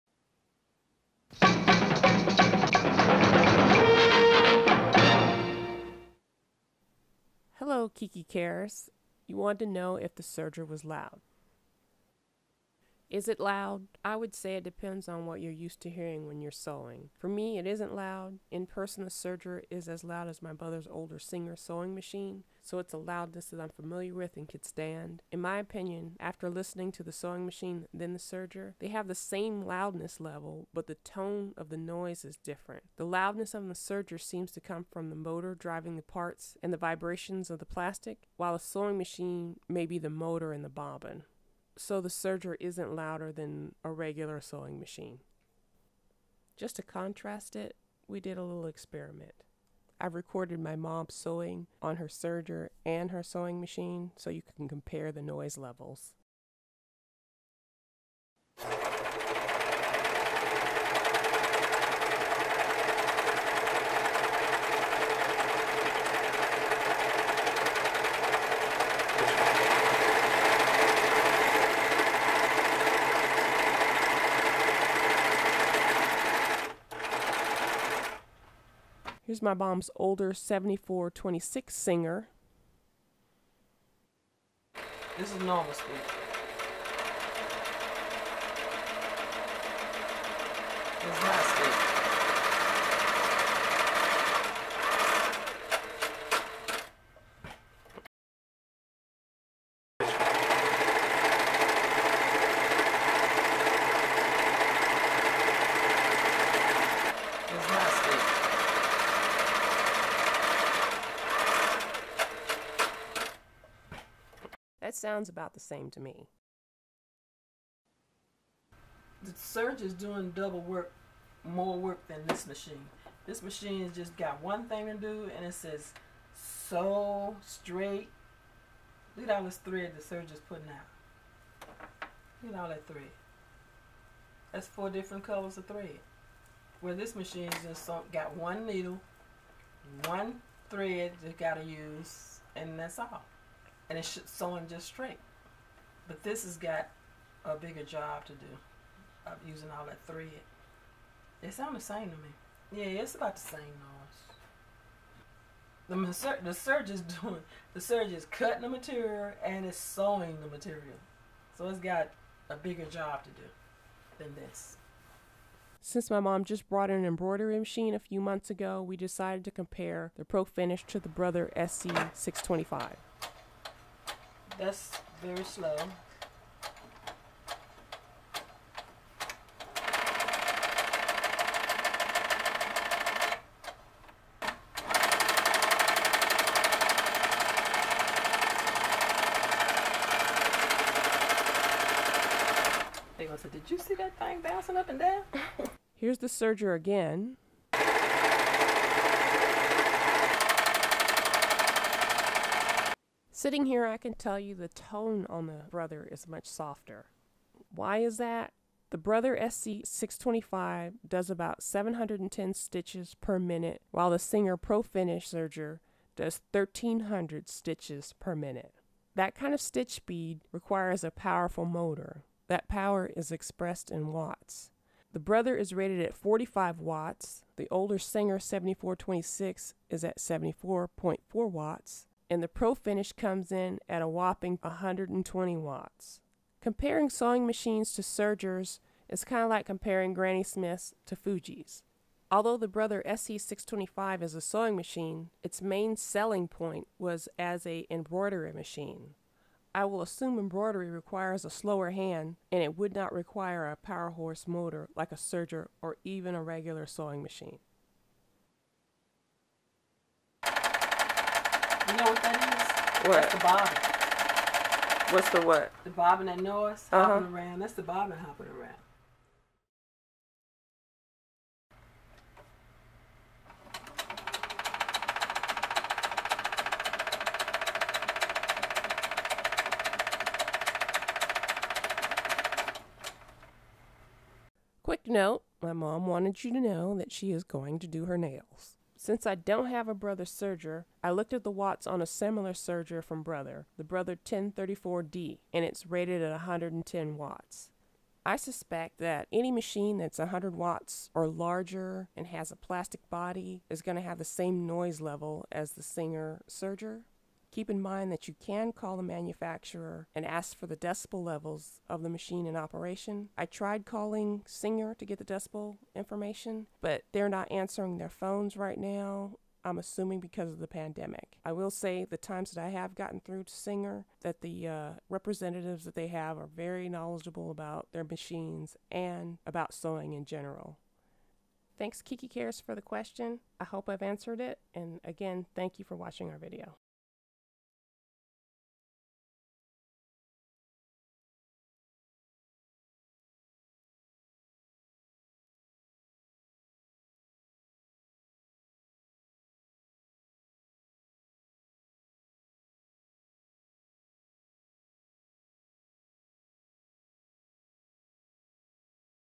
Is the 14CG754 Singer Pro Finish Serger Loud?
A exploration of the loudness of the Singer Pro finish 14CG754 Serger.